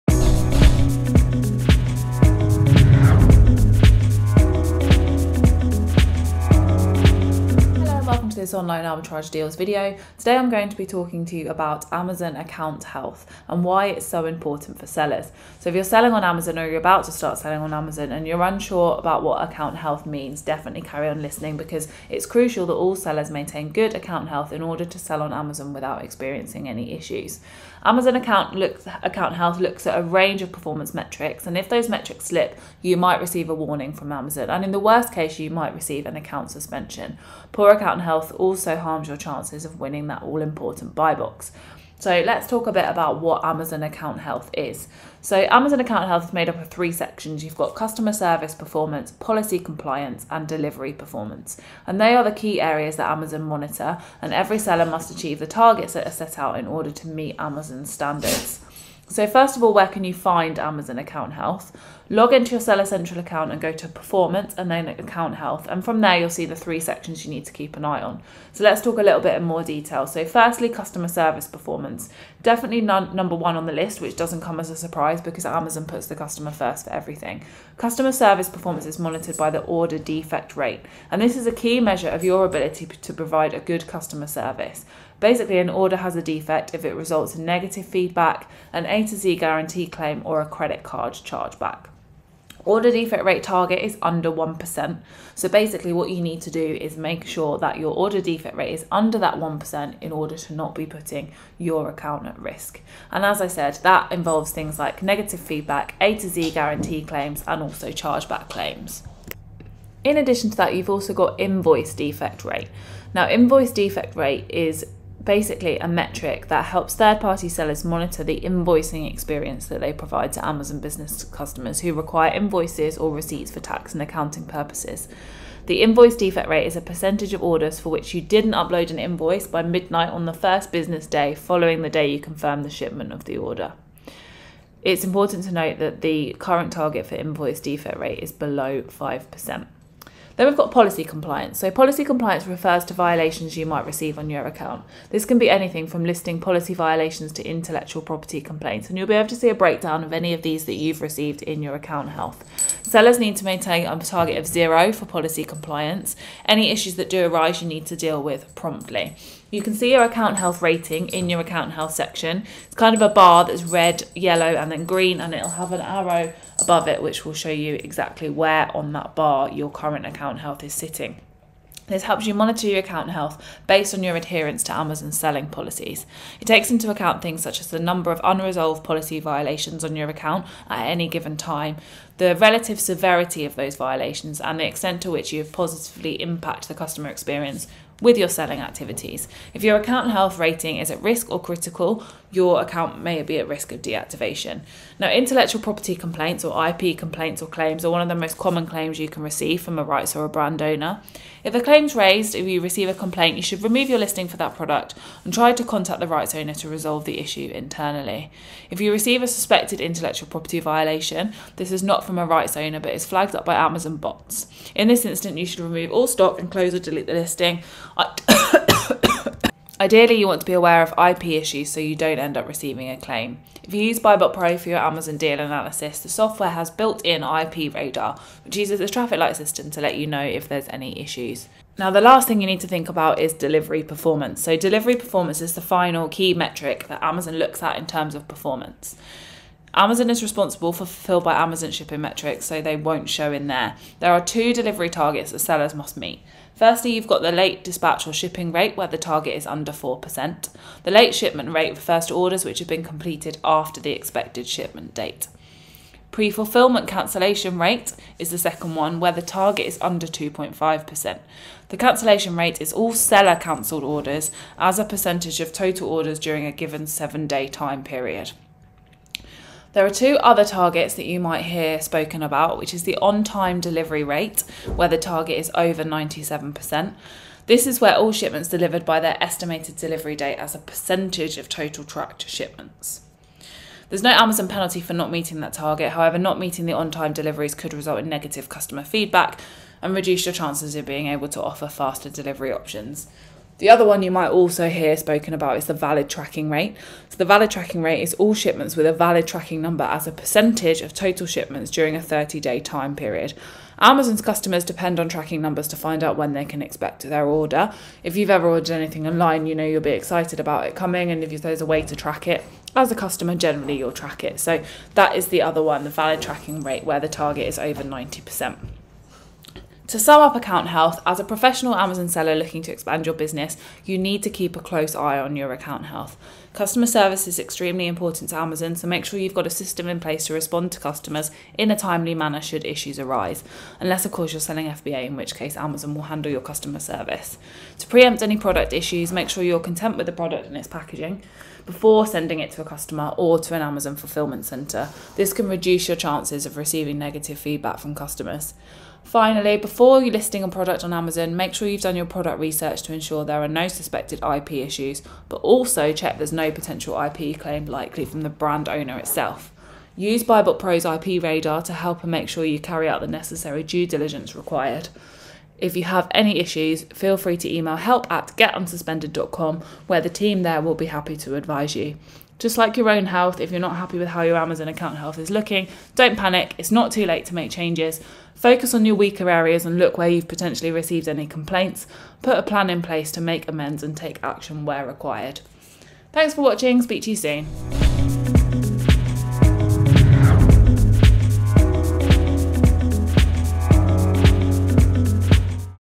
What Is Amazon Account Health And How Can You Ensure You Maintain Good Amazon Account Health?
When doing selling on Amazon it is so important to maintain good account health, many factors can contribute but listen on to hear the key factors!